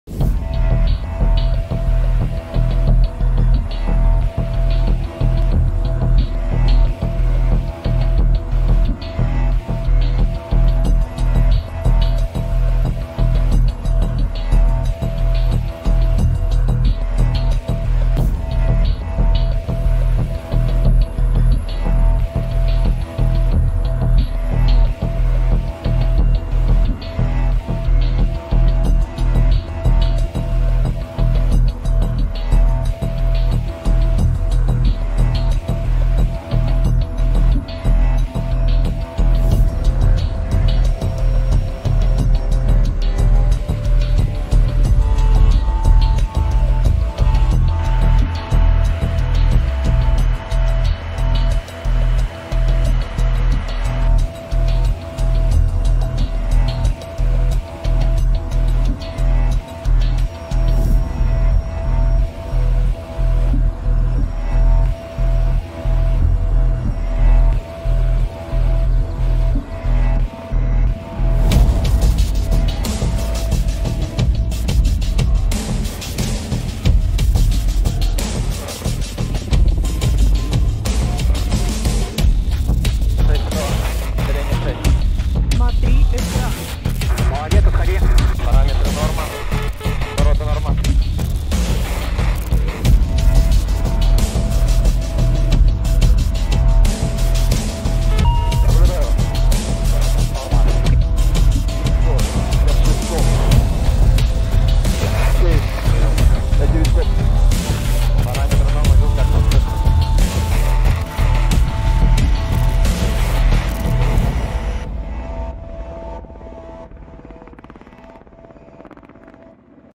Rosyjskie bojowe helikoptery likwidują ukraińskie stanowiska.
Rosyjskie Ministerstwo Obrony opublikowało materiał filmowy z działań bojowych przy obliczaniu przeciwpancernego systemu rakietowego „Kornet” jednostek Sił Powietrznych.
Załoga bezzałogowego statku powietrznego odkryła w opuszczonym przedsiębiorstwie zakamuflowane stanowisko ostrzału pojazdów opancerzonych Sił Zbrojnych Ukrainy.